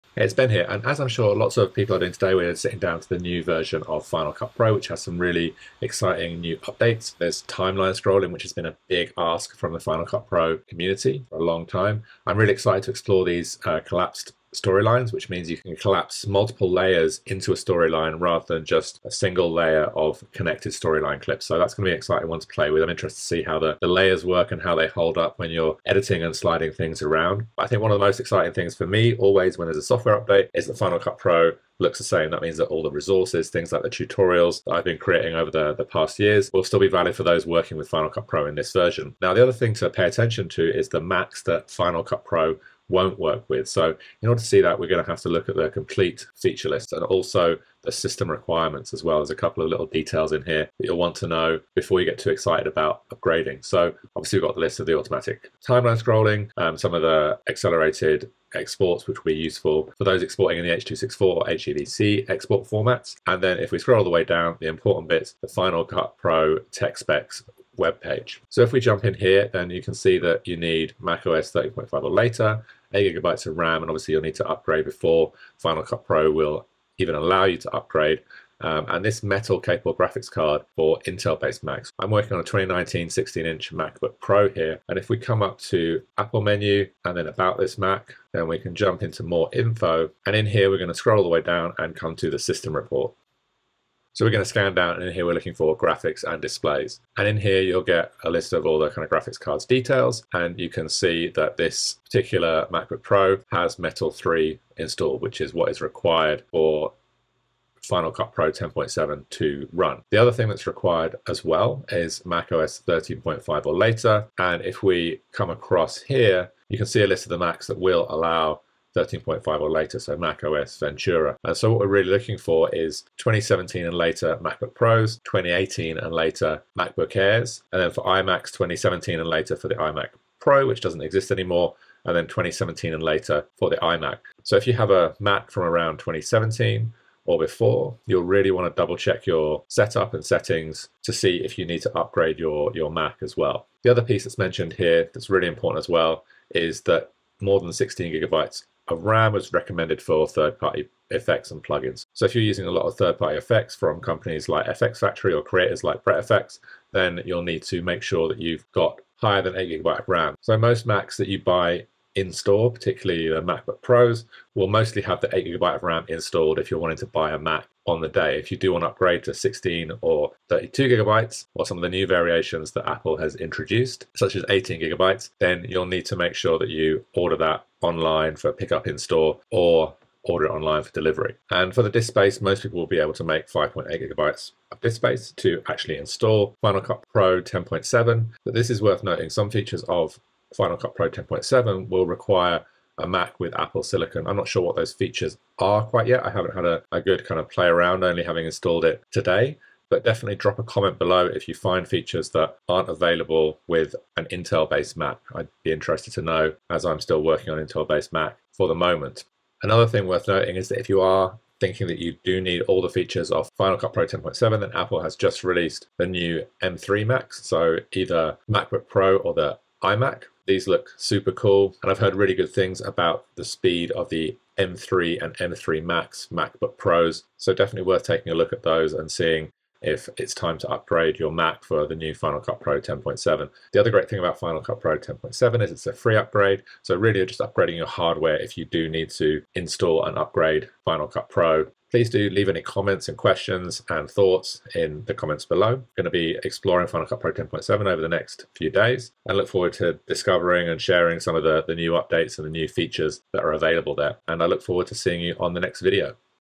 Final Cut Pro 10.7 | What is New & Is My Mac Compatible?
Learn how to find out if your Mac is compatible with Apple's new release of Final Cut Pro 10.7 in this tutorial.